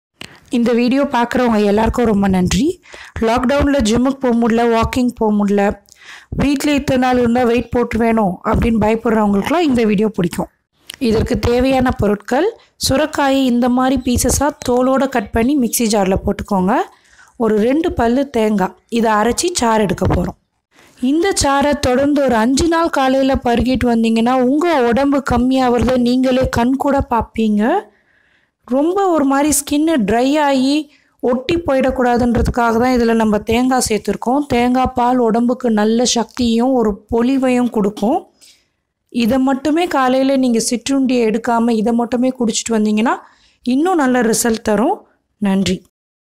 Lockdown weight loss drink with natural ingredient : Easy weight reducing drink .
Welcome to S cube samayal! In this video you can make weight-reducing juice at home.